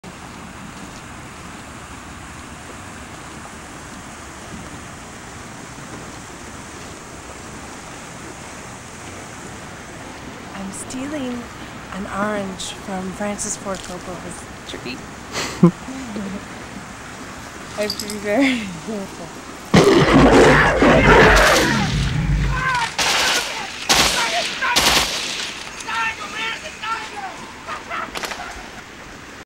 Stealing an orange from Francis Ford Coppola's tree
Stealing an orange from Francis Ford Coppola's tree. Coppola winery, Napa Valley.